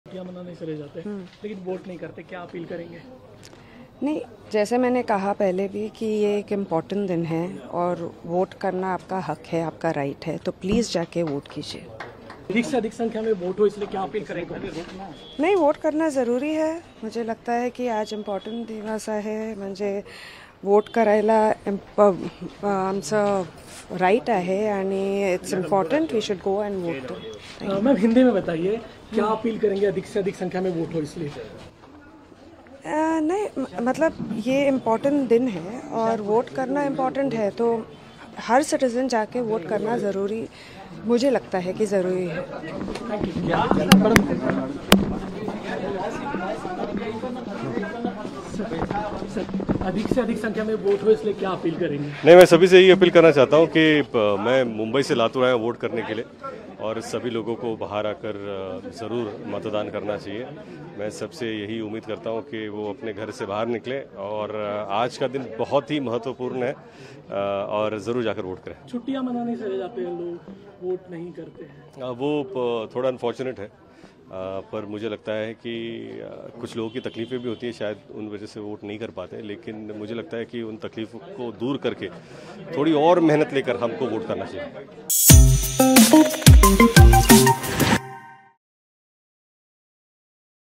Lok Sabha Election Phase 3 Voting: Riteish Deshmukh और Genelia ने लातूर के मतदान केंद्र में वोट डाला
Lok Sabha Election Phase 3 Voting: महाराष्ट्र: अभिनेता रितेश देशमुख और उनकी पत्नी जेनेलिया देशमुख ने लातूर के एक मतदान केंद्र पर वोट डालने पहुंचे। NDA ने मौजूदा सांसद सुधाकर तुकाराम श्रंगारे और INDIA गठबंधन के कलगे शिवाजी बंदप्पा को मैदान में उतारा है।